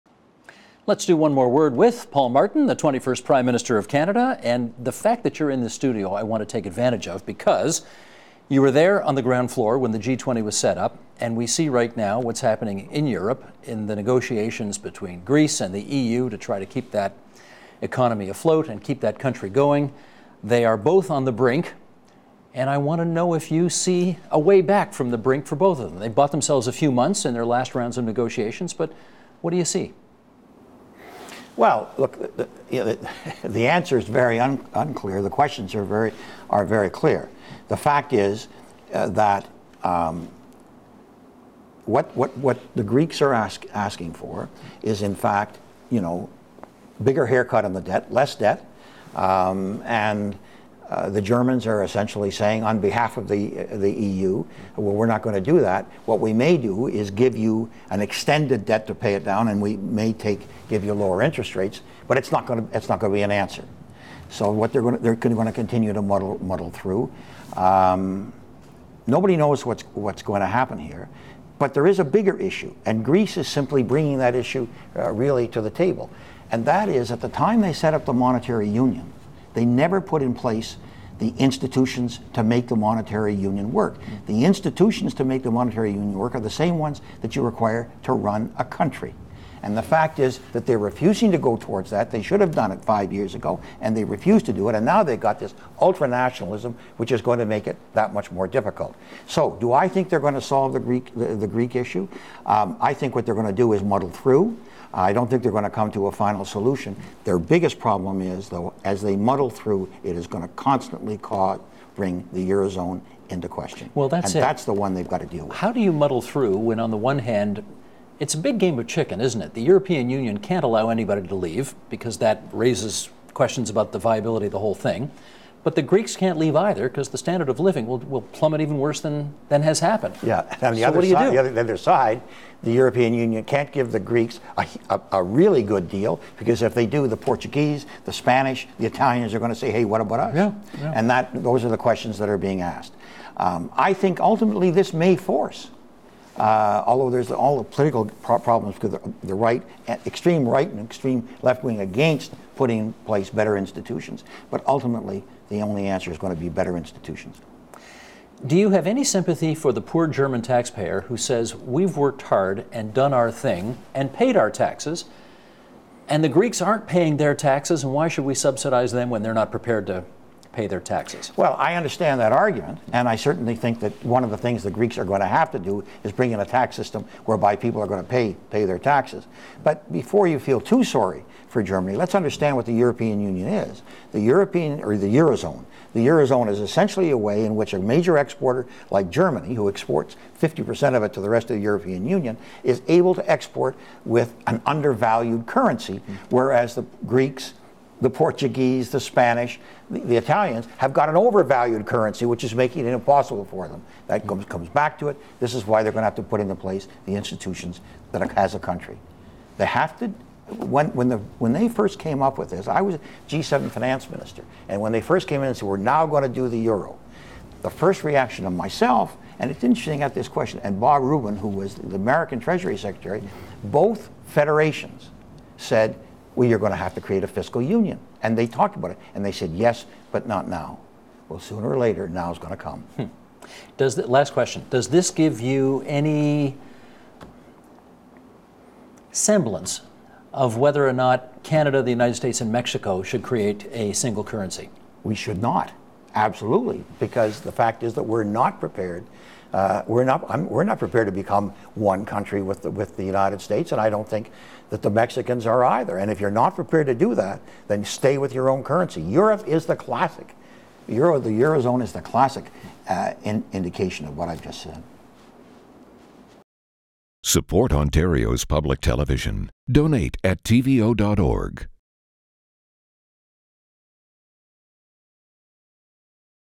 Agenda Plus: Paul Martin
The 21st Prime Minister of Canada, Paul Martin, on Greece's debt, the bailout extension, and the European Union.